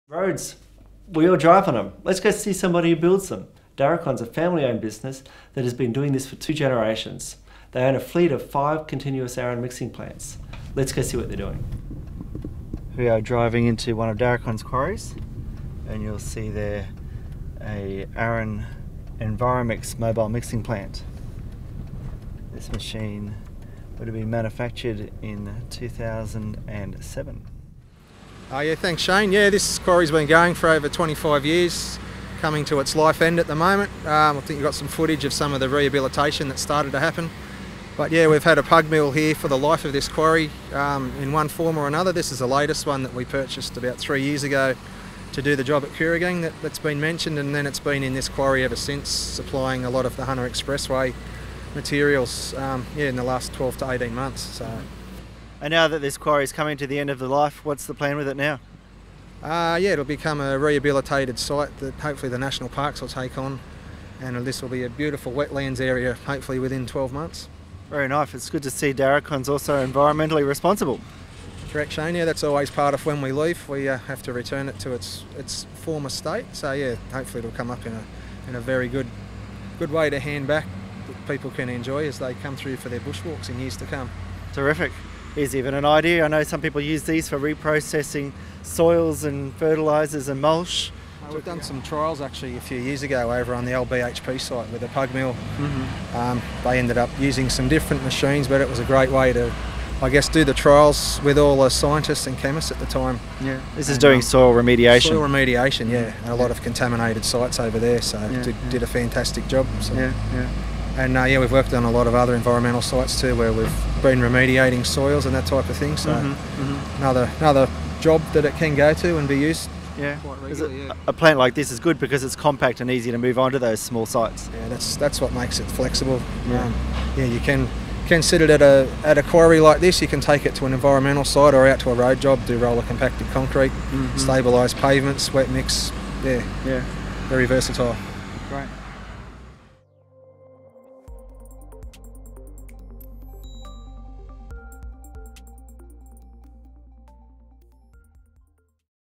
What can you do with an Aran Mobile Mixer? Hear from someone with 5 of them
Take a look inside a Daracon quarry featuring the Aran Environmix, hear about road jobs, roller compacted concrete, stabilised pavements, wet mix, bio remediation and the Hunter Expressway.

for the aerial photo of the Hunter Expressway (1:30)